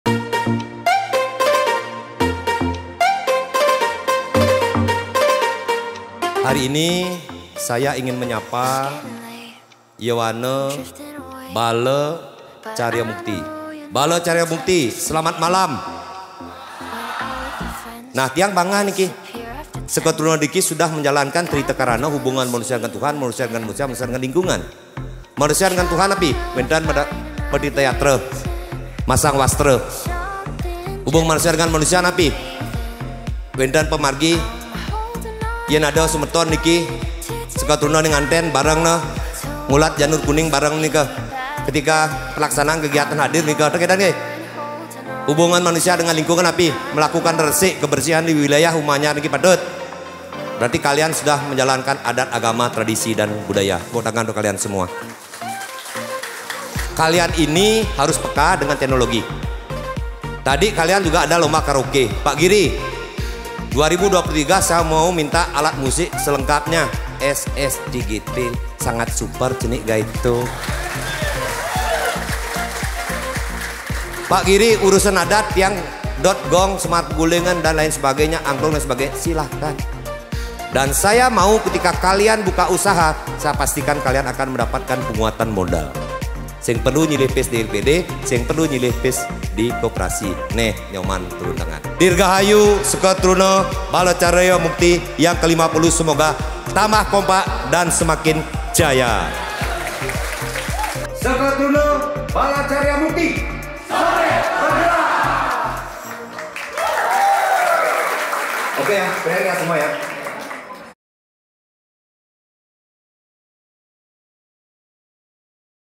Bupati Badung Hadiri Puncak Perayaan HUT ST Bala Carya Mukti ke-50
Bupati Badung Giri Prasta komit mendukung kegiatan Sekaa Teruna yang ada di wilayah Kabupaten Badung, hal itu tampak saat menghadiri puncak perayaan HUT ST Bala Carya Mukti ke-50 di Wantilan Pura Desa Adat Umahanyar, Abiansemal, Jumat (23/9) malam.  Acara turut dihadiri anggota DPRD Bali I Bagus Alit Sucipta, DPRD Badung I Made Ponda Wirawan, Camat Abiansemal IB. Putu Mas Arimbawa dan unsur Tripika Abiansemal, Bendesa Adat Umahanyar I Ketut Nuridja, Ketua ST Bala Carya Mukti I Gede Yogi Antara Putra, Ketua Panitia I Gede Surya Adeputra serta undangan lainnya.

Puncak acara HUT ST Bala Carya Mukti ditandai dengan pemotongan tumpeng yang langsung dilakukan oleh Bupati Giri Prasta dan diserahkan kepada Ketua ST Bala Carya Mukti. Pada kesempatan tersebut, Bupati Giri Prasta juga turut menyerahkan bantuan Rp. 15 juta yang bersumber dari dana pribadi.